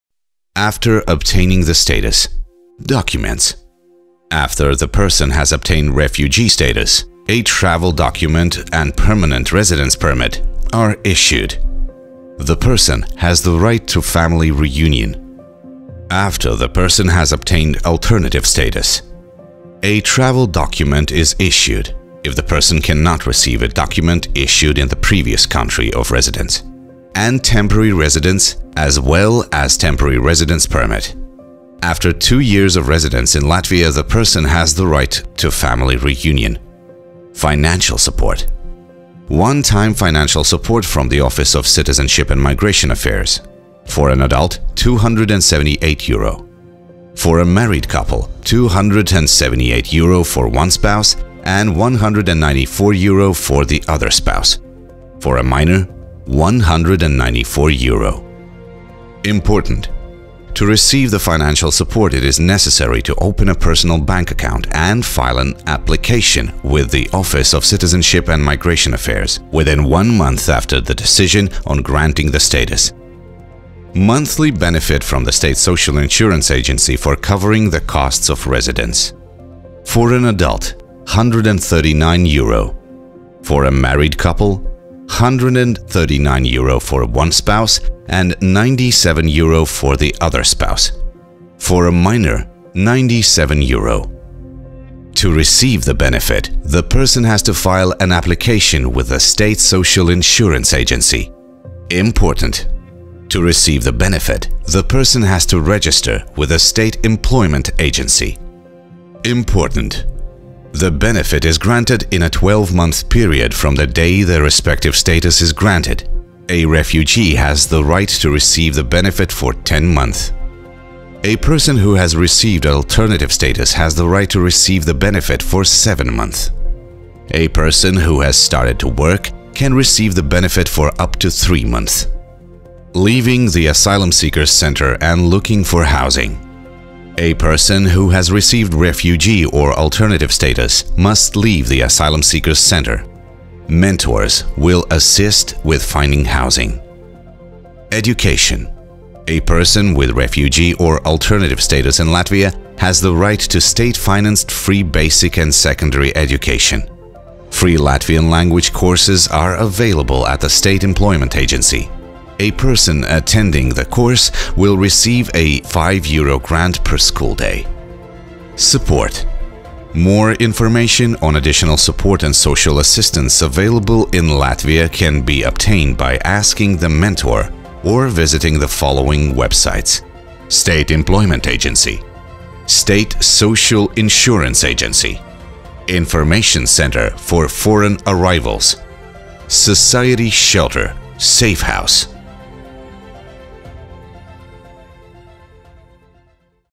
What are the rights and obligations of an asylum seeker after being granted status
Watch the video and find out:
👉what documents can be issued after obtaining refugee status
👉about financial support
👉about the place of residence
👉where to look for extra support

Chapters:
00:00 - Documents after obtaining refugee status
00:36 - Financial support 1
00:59 - Important!
1:13 - Financial support 2
1:49 - Important!
2:13 - Leaving the asylum seekers center and looking for housing
2:27 - Education